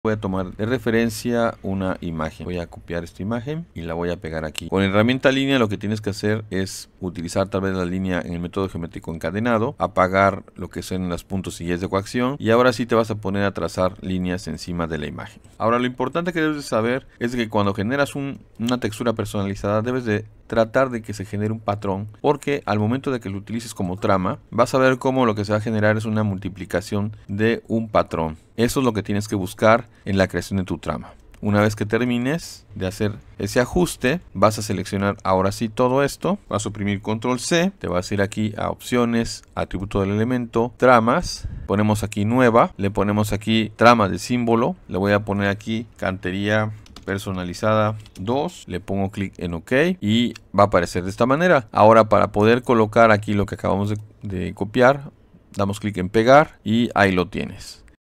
Archicad Como Crear una trama
Aprende como crear una trama personalizada en archicad